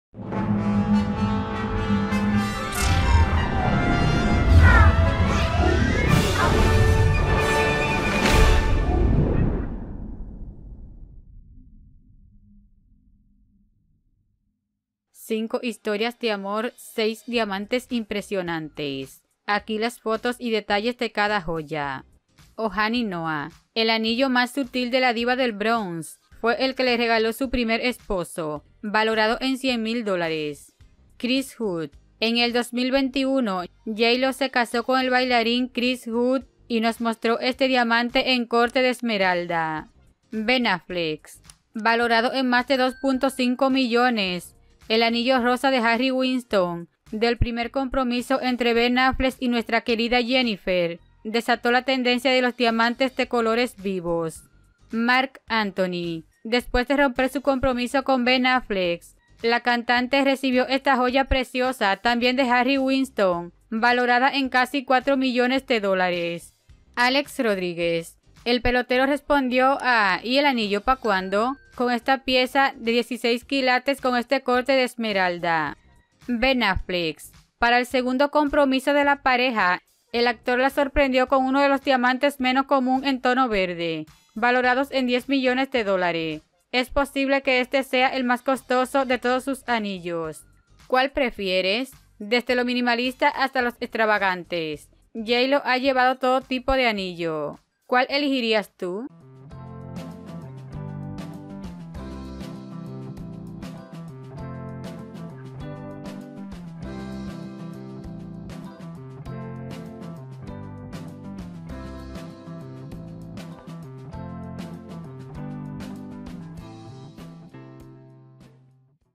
¿Cuál ha sido el mejor ANILLO de COMPROMISO de Jennifer López?
¡Cinco historias de amor, seis diamantes impresionantes! Aquí las fotos y detalles de cada joya.